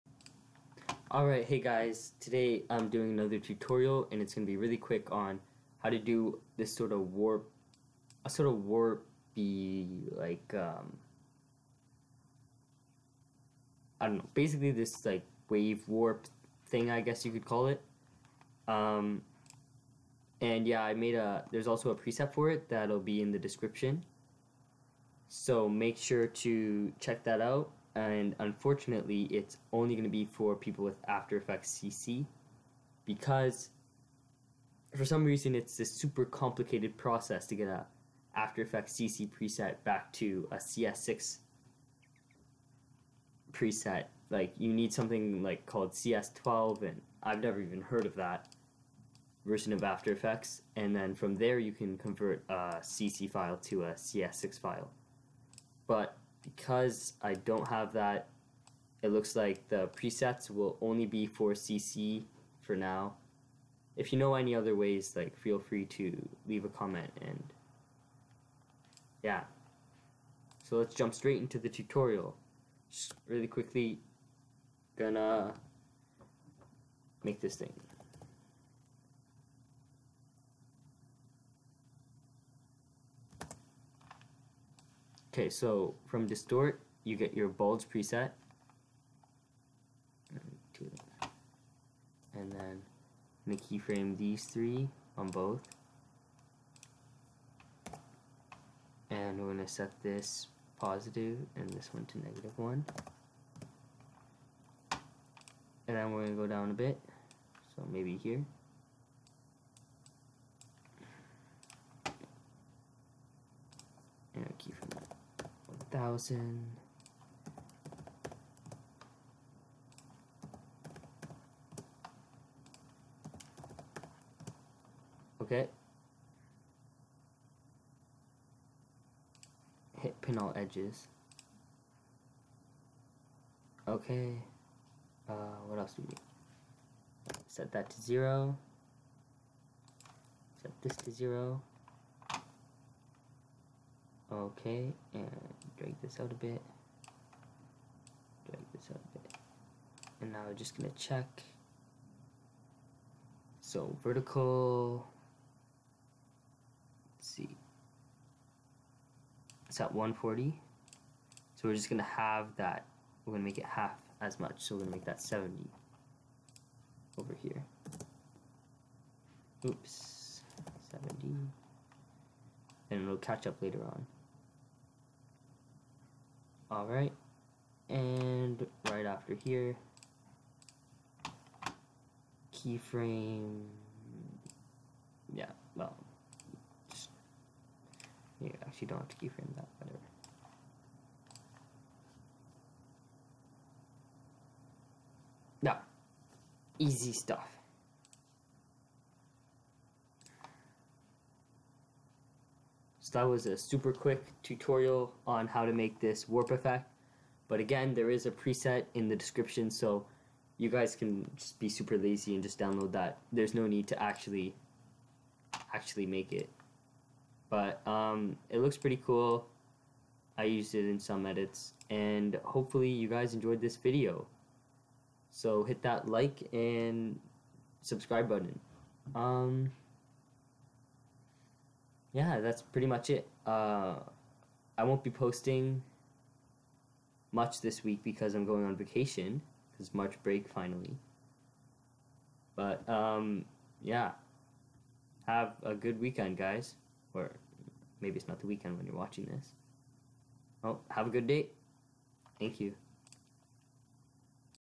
After Effects Bulge Warp [Free Preset/Tutorial]
Hey guys new tutorial about a warp bulge effect, preset working for after effects CS6+. Hope it helps :)